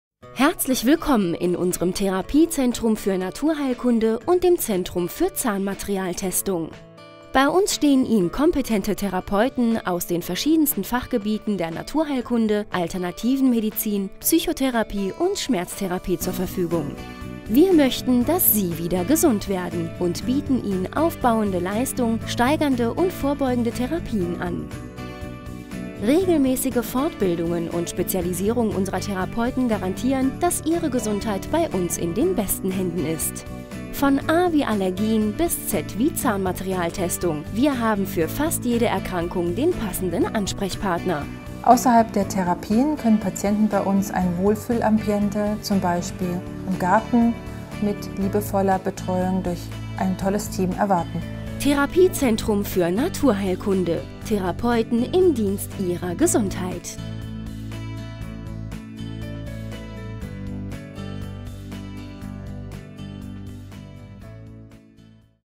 Therapiezentrum Mannheim
Wir arbeiten seit 1994 u.a. im Bereich der Naturheilkunde, orthemolekularen Medizin (=Hochdosis Mineralstoff und Vitamintherapie), der Allergiebehandlung, Pflanzenheilkunde, Störfeldbeseitigung und der manuellen Therapie.